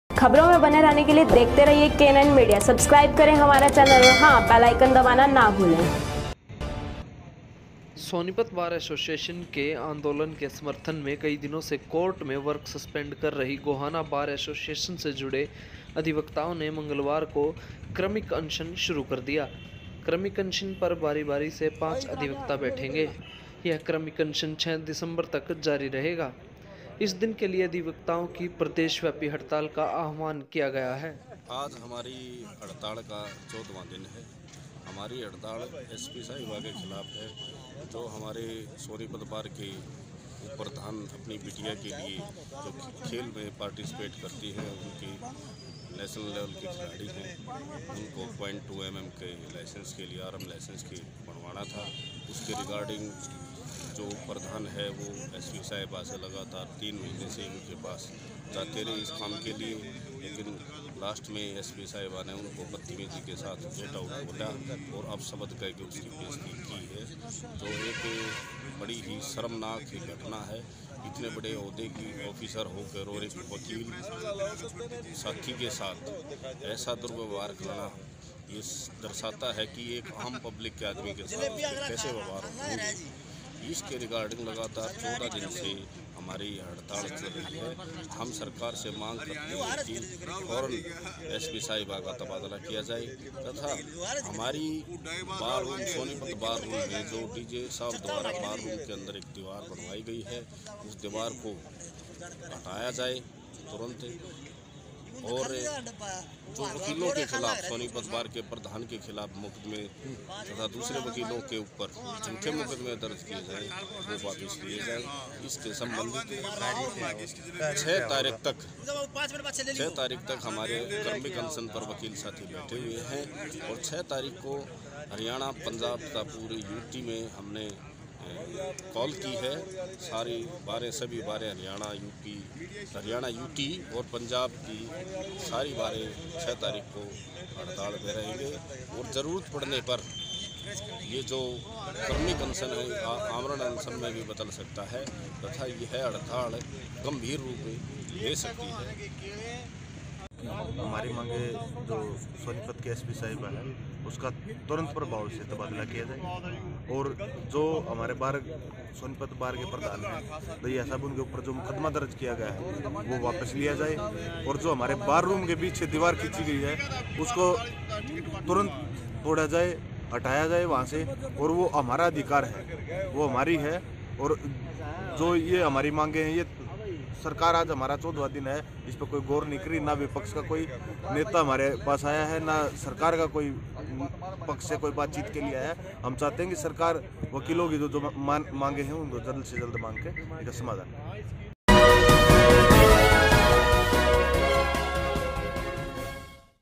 SP के तबादले को लेकर वकीलों जंग तेज़
गोहाना बार एसोसिएशन के वकील बैठे कृमिक अनशन पर
Gohana bar association advocates